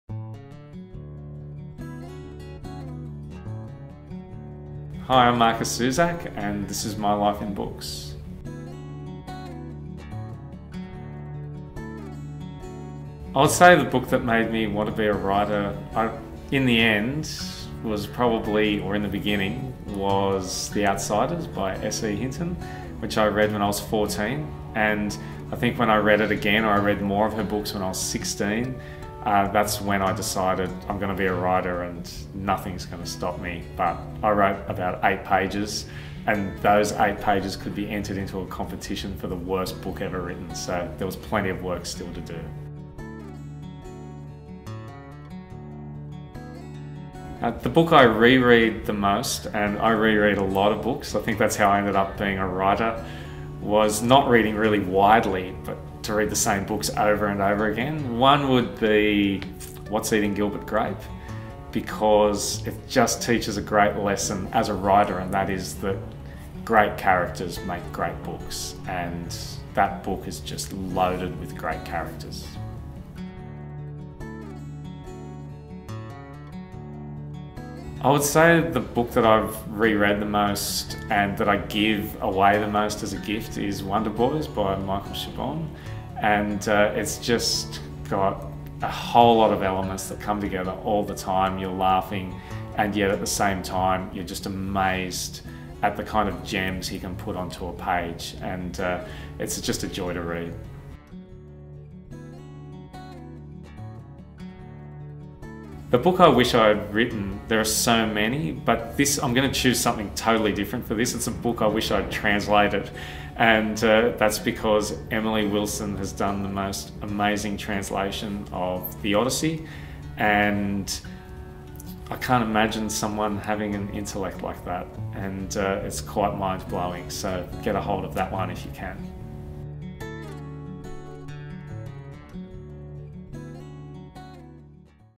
My Life in Books: Markus Zusak
Markus Zusak is the international bestselling Australian author of The Book Thief, When Dogs Cry, and The Messenger. He is now back with Bridge of Clay.